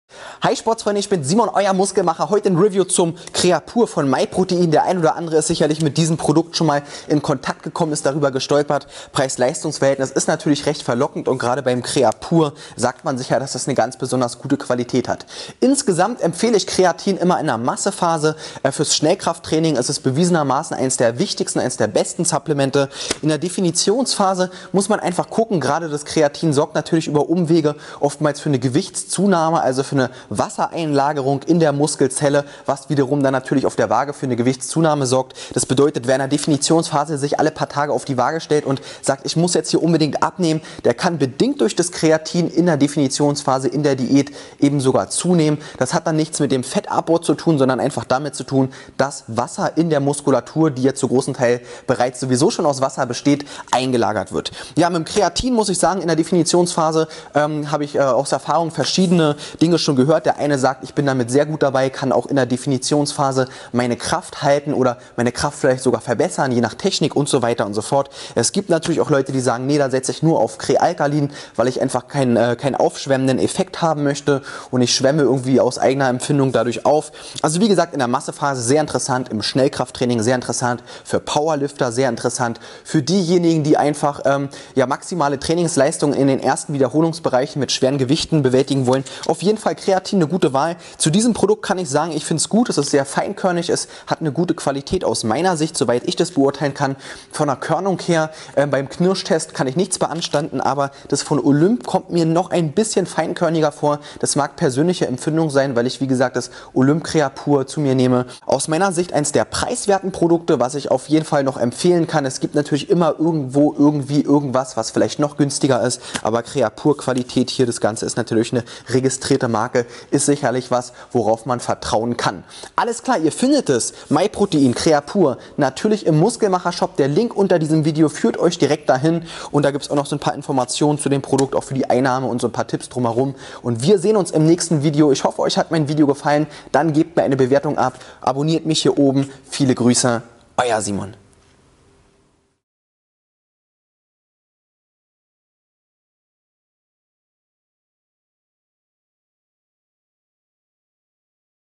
MyProtein Creapure Creatin Monohydrat
Das Creapure Creatine Monohydrate von My Protein gilt als ein hochwertiges Creatinpulver. In diesem Video erkläre ich wofür es verwendet wird und wem es was nützt.